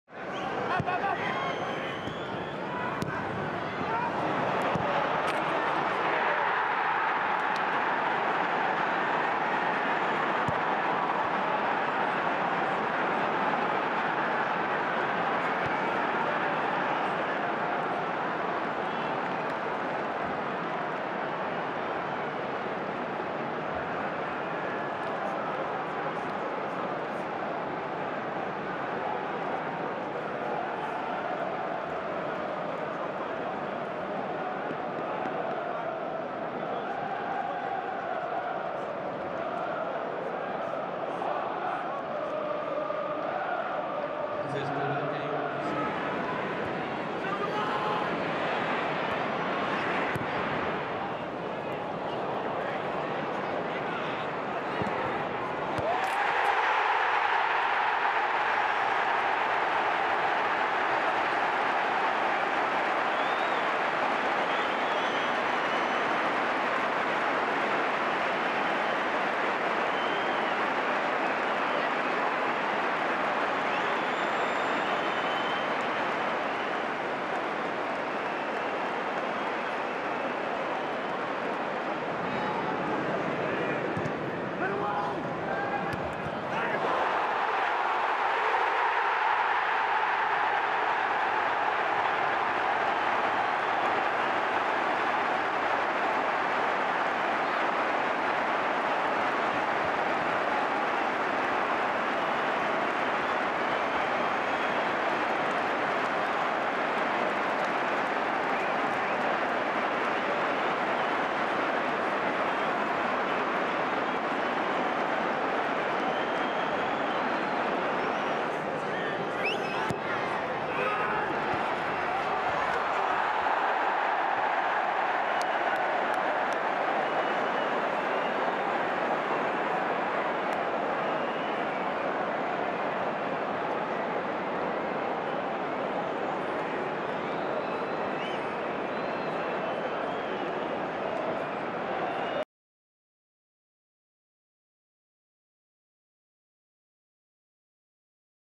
DEFEAT AT NEWCASTLE 😞 | Newcastle 2-1 AFC Bournemouth
The short highlights as AFC Bournemouth fell short 2-1 to Newcastle on the road.